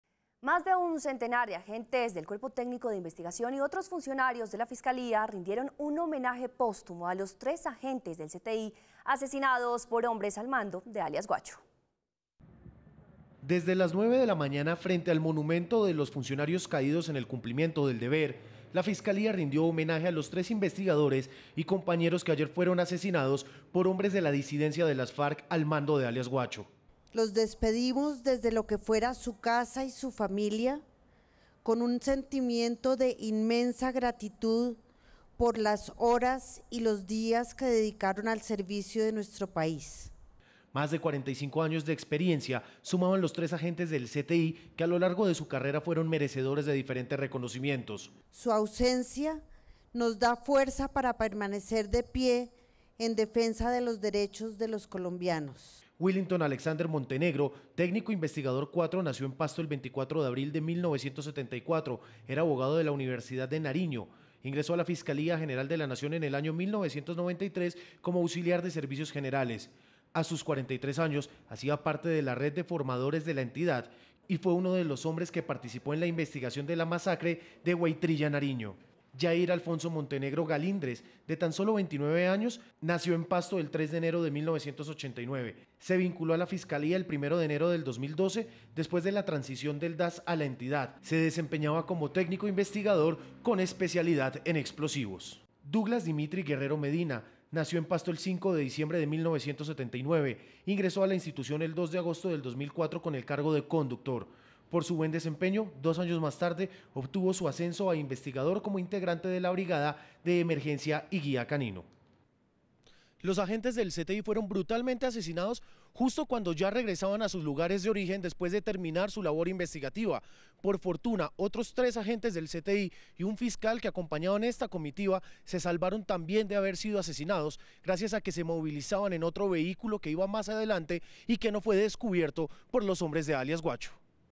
RED+ | Fiscalía rindió homenaje a agentes del CTI asesinados por alias "guacho"
Más de un centenar de agentes del cuerpo técnico de investigación y otros funcionarios de la fiscalía, rindieron un homenaje póstumo a los tres agentes del CTI, asesinados por hombres al mando de alias guacho